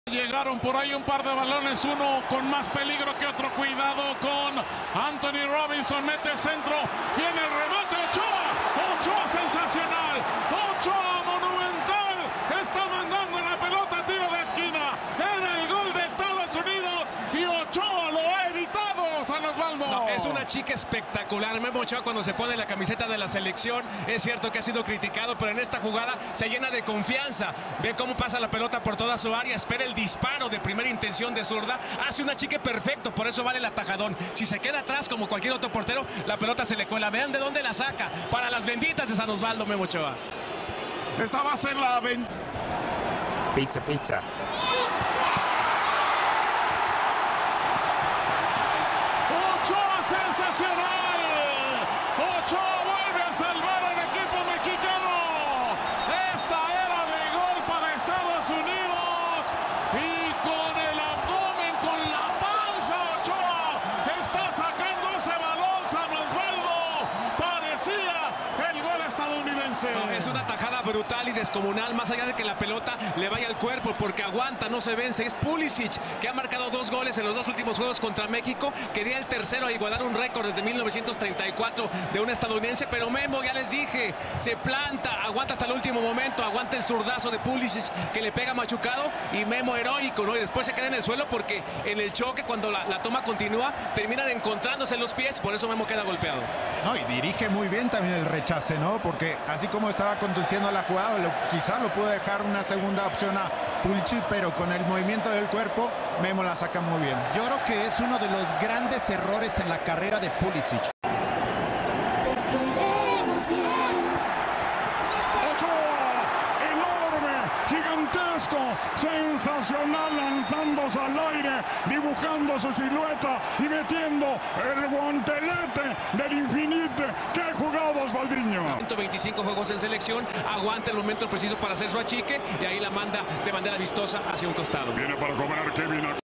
Seguimiento | Ochoa y sus grandes atajadas | México 0-0 Estados Unidos | Eliminatoria Qatar 2022
El portero del Tri tuvo salvadas espectaculares, sobre todo ante Pulisic, que permitieron el empate.